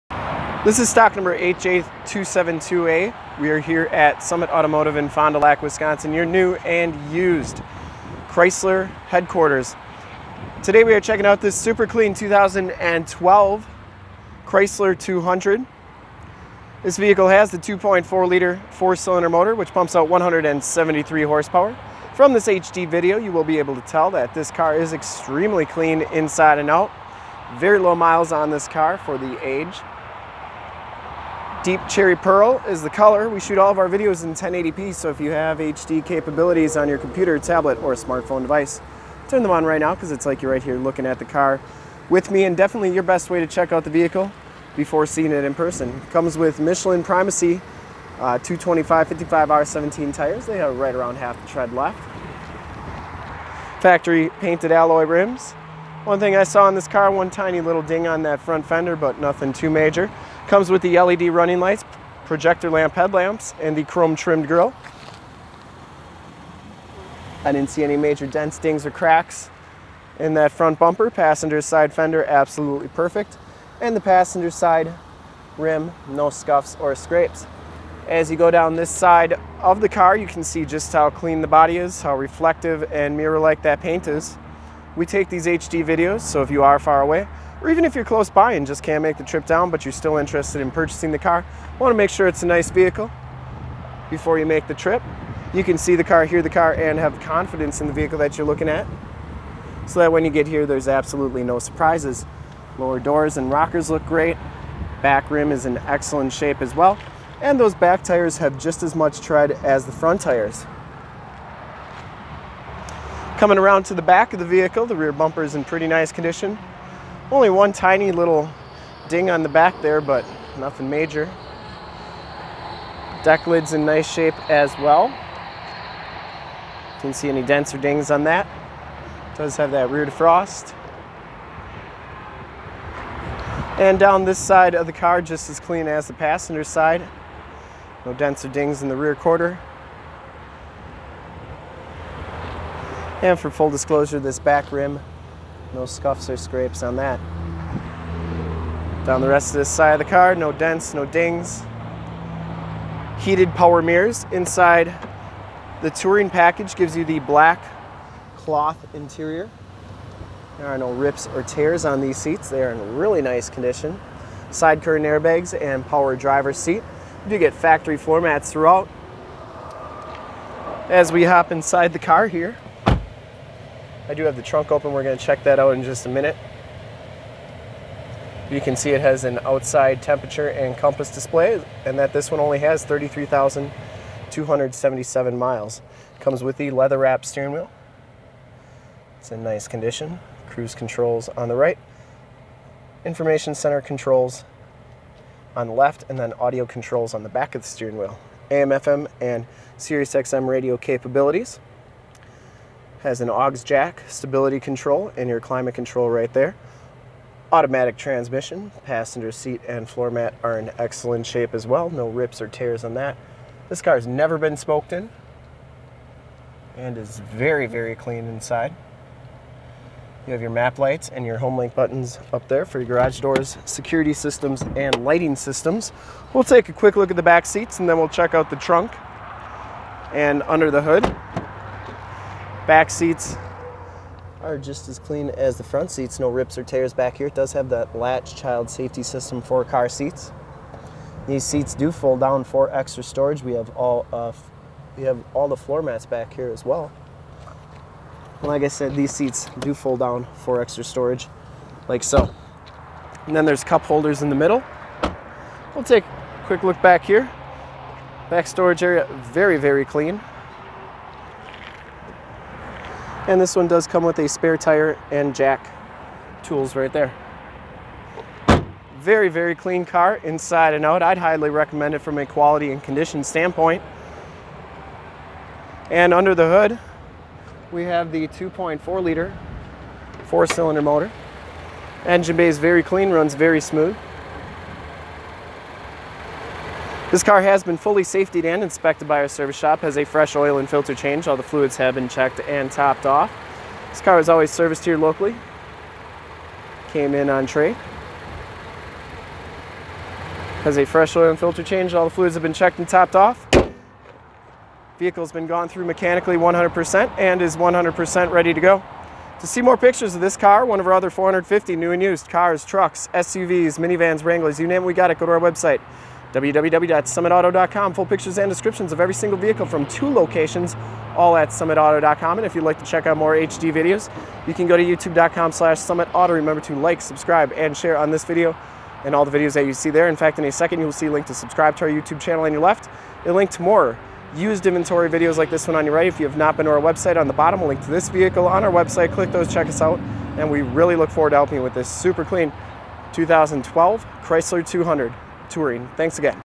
2012 CHRYSLER 200 TOURING DEEP CHERRY RED FOND DU LAC OSHKOSH WISCONSIN SOLD! 8J272A
This 2012 CHRYSLER 200 TOURING IN DEEP CHERRY is the vehicle we did walk around review of today.

2012 CHRYSLER 200 TOURING IN DEEP CHERRY

STOCK: 8J272A
PRICE: $10,499
MILES: 33,253
MAKE: CHRYSLER
MODEL: 200
VIN: 1C3CCBBB1CN278769
LOCATION: FOND DU LAC OSHKOSH WISCONSIN, 54937 TRUCKS ON 41

1 OWNER! CLEAN TITLE HISTORY! 2.4 Liter I4 DOHC Engine, 173 Horsepower, Four Door Sedan, Touring Package, 6 Speed Automatic Transmission, Automatic Transmission Center Console Shifter, Front Wheel Drive FWD, Power Driver's Seat, Non Smoker, Black Ebony Cloth Seats, Bucket Seats, Heated Power Mirrors, Electronic Stability Control Traction Control ESC, Michelin Primacy HX MXM4 225/55 R17 Tires, Factory Alloy Rims Premium Wheels, Four Wheel Disc Brakes, LED Running Lights, Projector Lamp Headlights, Chrome Trimmed Grill, AM / FM Radio Tuner, Sirius/XM Satellite Radio Capabilities Sirius / XM, CD Player, Auxiliary MP3 Jack Portable Audio Connection, Keyless Entry System, Rear Window Defroster, Adjustable Height Seatbelts, Driver and Passenger Front Air Bags, L.A.T.C.H. Child Safety System, Side Curtain Air Bags SRS Safety Restraint System, Multi-Function Steering Wheel Controls, Homelink System with Three Programmable Buttons for Garage Doors, Lighting Systems & Security Systems, Compass, Outside Temperature Display and Mileage Display, Fold Down Rear Seats, Factory Floormats, Air Conditioning AC, Cruise Control, Power Locks, Power Windows, Automatic Headlights Autolamp, Tilt/Telescope Steering Wheel, Deep Cherry Red Crystal Pearlcoat, ONE OWNER! CLEAN AUTOCHECK! Very very clean inside and out! This is one of the sharpest 2012 Chrysler 200 cars we have ever had on our lot! Make your move before this super clean ride is gone!

STOCK: 8J272A
PRICE: $10,499
MILES: 33,253
MAKE: CHRYSLER
MODEL: 200
VIN: 1C3CCBBB1CN278769